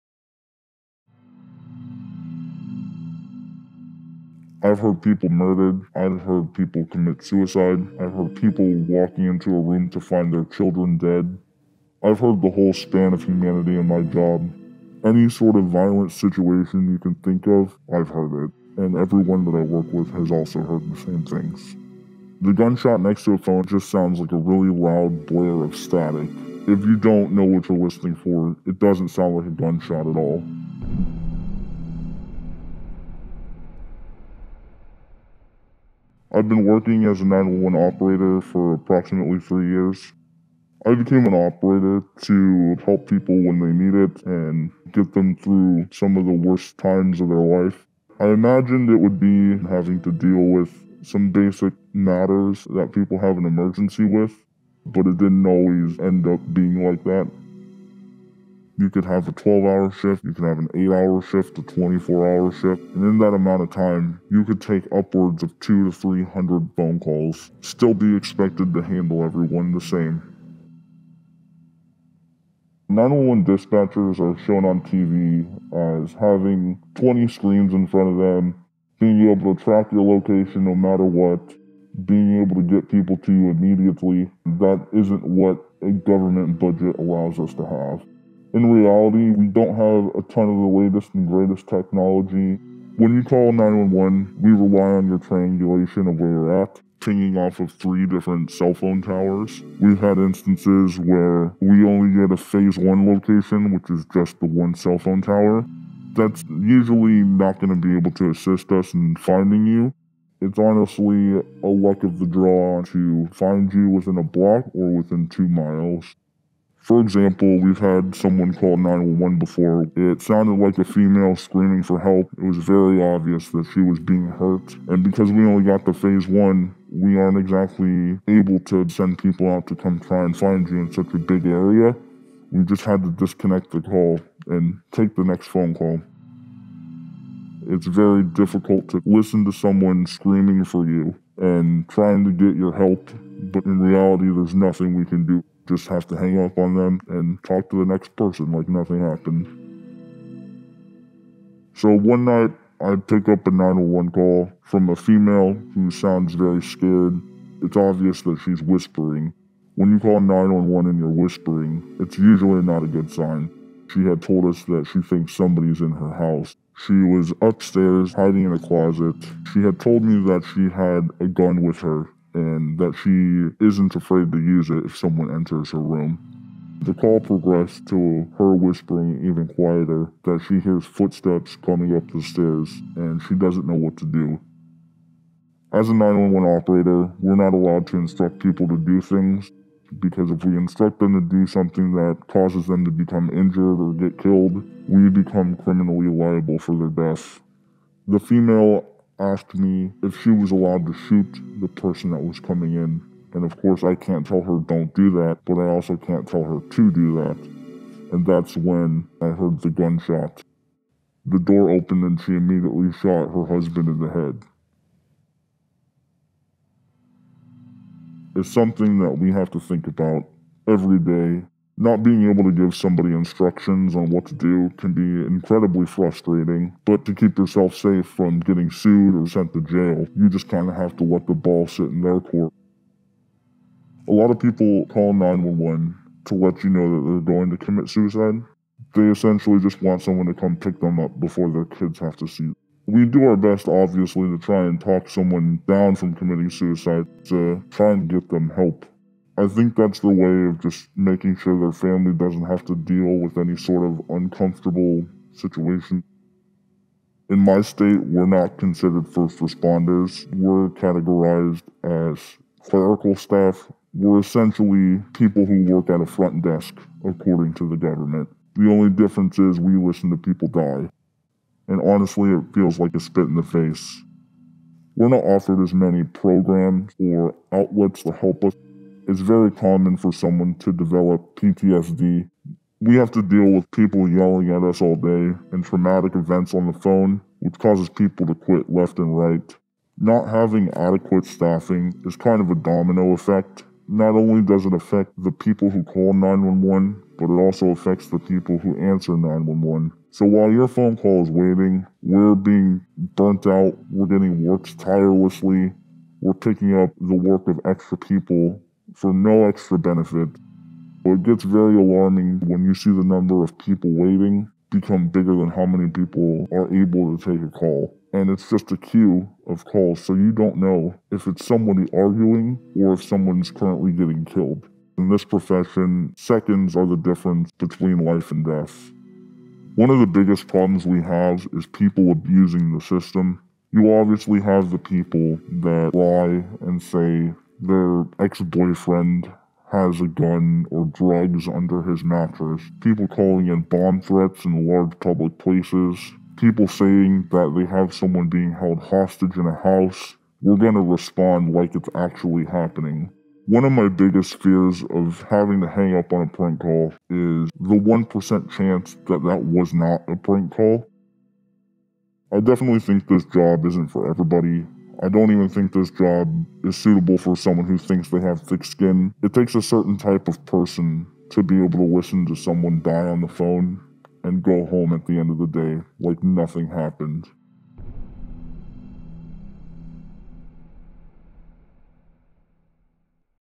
The Horrors You Hear as a 911 Operator | Informer
“I have heard people murdered. I have heard people commit suicide. I have heard people walk into a room to find their children dead.”

These are just some of the daily horrors that the Informer has to deal with, day in, day out. A thankless task, listening to the very worst of humanity, on 24-hour rotations, fielding up to 300 calls per shift.

Having worked as a 911 operator and dispatcher for the past four years. Thos Informer knows all too well the secret reality of answering your 911 calls.

He describes how the lack of modern technology means people have to be hung up on, even if they are screaming for help. Or how, they are unable to tell callers what to do, for fear of being sued - often with deadly consequences.

All the while, they are not considered front-line workers, and with that, none of the protections or benefits.  “We are essentially people at a front desk, according to the government - the only difference is, we listen to people die.”

Informer is a series where we hear the anonymised confessions and insights of people on the inside of different industries, institutions and events.